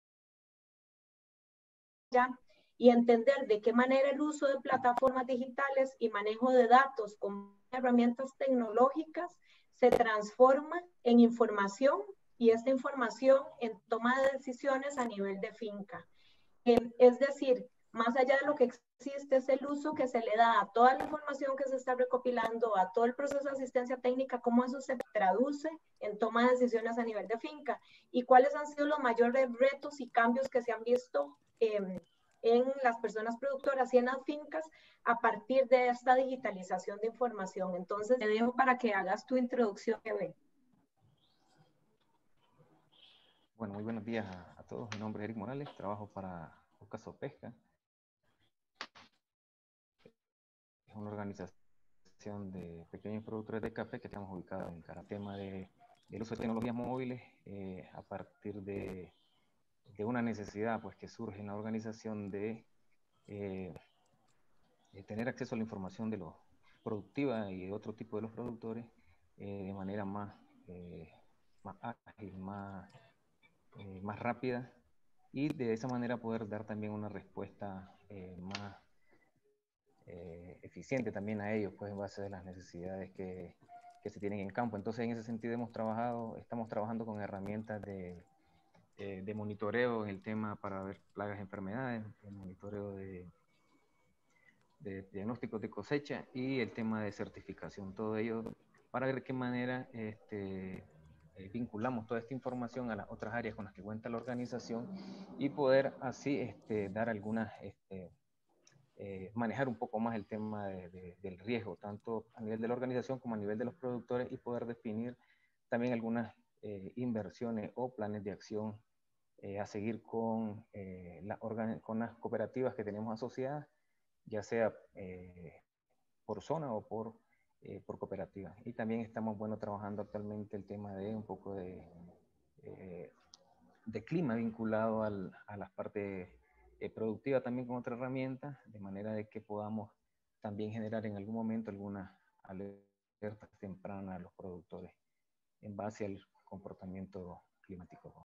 ¿Cómo los datos se traducen en toma de decisiones en fincas?: Erick Antonio Morales Rivera
Experiencias de Origen: Integrando Herramientas Digitales| Somos Digital Coffee Future. Te invitamos a aprender más de las sesiones que tuvieron lugar entre el 18 de Noviembre y 2 de Diciembre de 2020.

⌚CONVERSACIÓN DURANTE LA TERCERA SESIÓN: El uso de herramientas digitales para cooperativas y productores.
Moderadora: ARIANA ARAUJO RESENTERRA - OFICIAL DE PROYECTOS DE LA PLATAFORMA SAFE
Panelista: ERICK ANTONIO MORALES RIVERA - COORDINADOR TÉCNICO SOPPEXCCA.

Erick responde a la pregunta de introducción: ¿Cómo los datos se traducen en toma de decisiones en fincas?

🙋‍♂️🙋 ¿Te gustaría conversar con nosotros? Completa el siguiente formulario

📂 Puedes conocer más detrás de la iniciativa en nuestra página web y nuestro canal oficial en YouTube